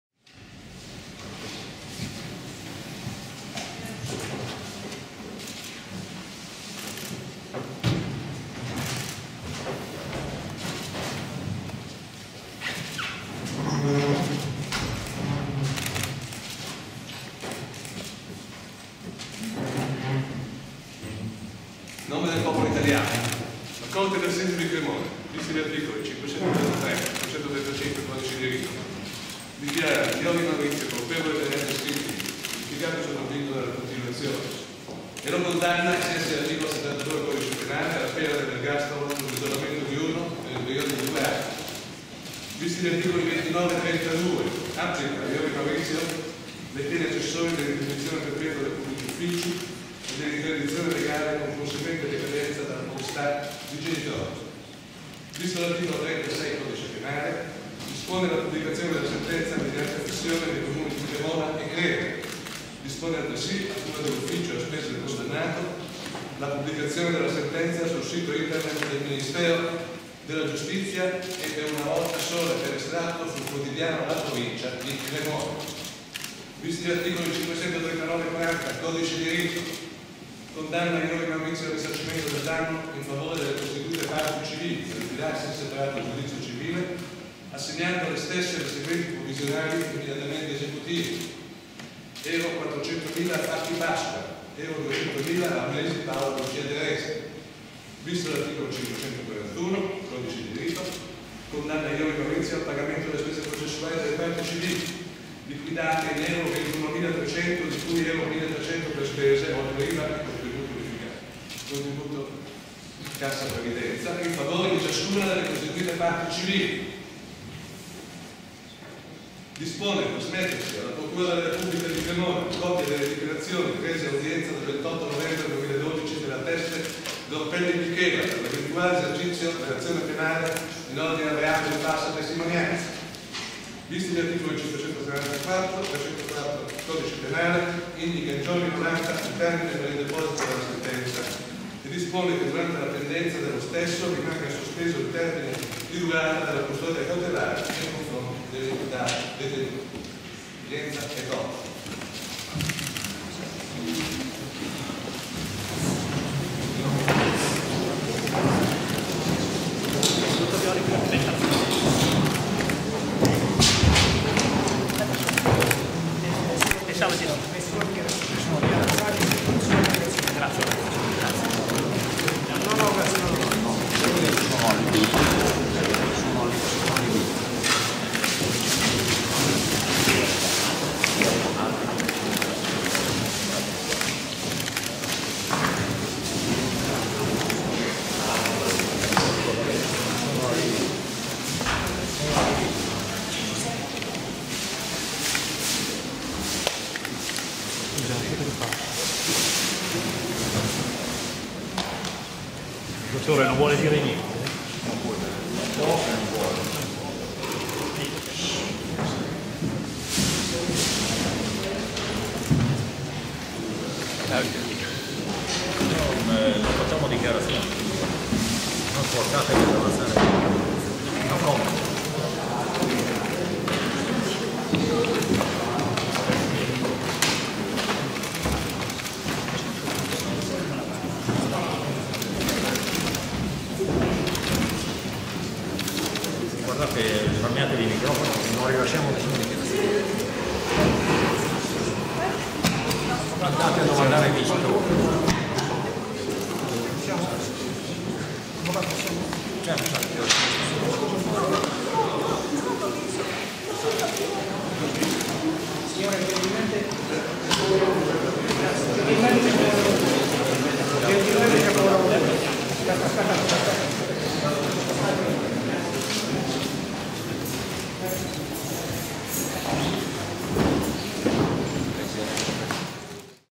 L'oculista Maurizio Iori condannato all'ergastolo per duplice omicidio
La Corte d'assise di Cremona condanna Maurizio Iori, oculista di Crema, all'ergastolo per l'omicidio di Claudia Ornesi e della figlia Livia